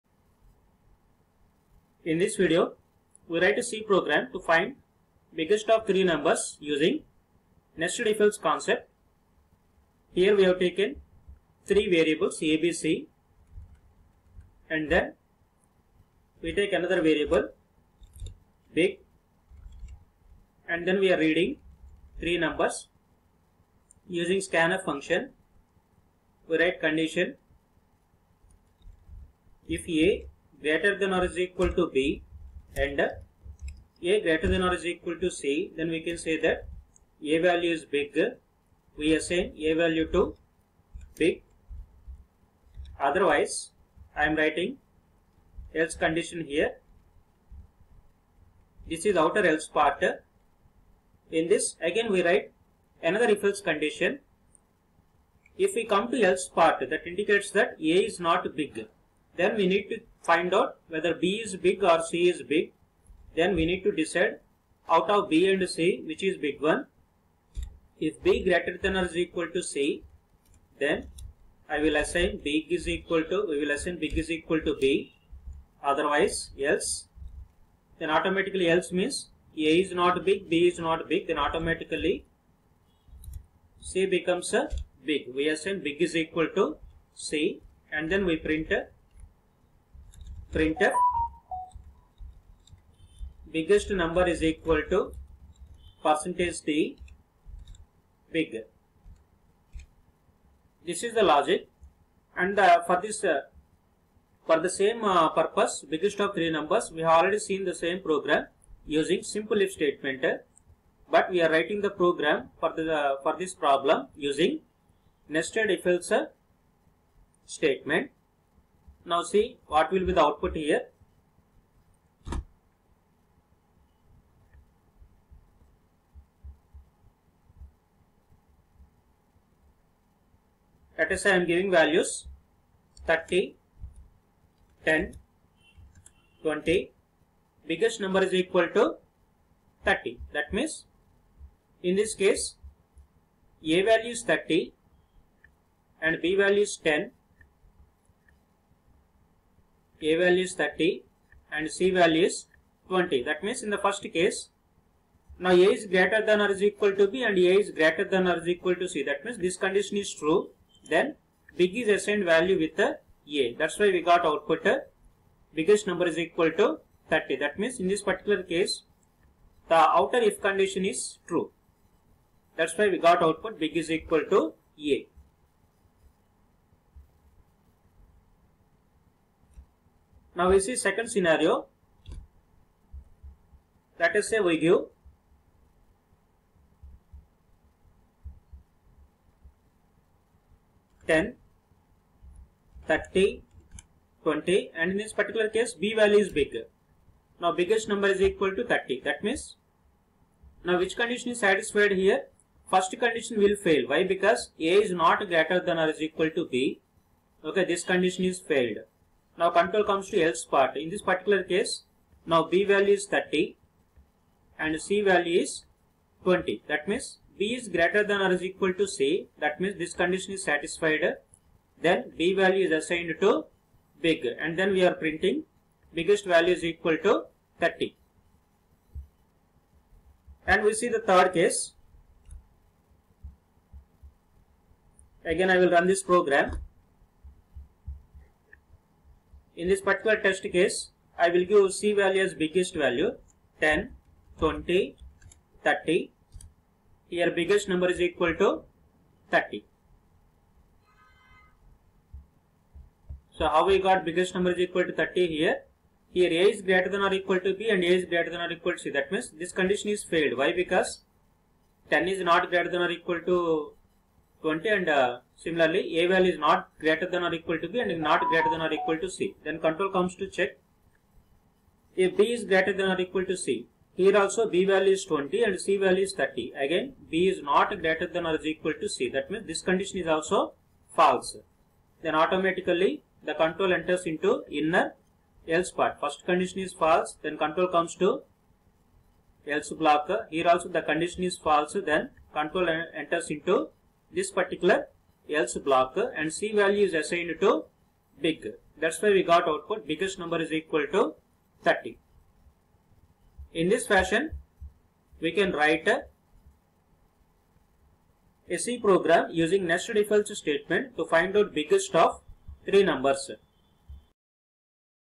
C program to find biggest of 3 numbers using nested if else statement
next video topic:  C program to find a year is leap year or not using nested if else statement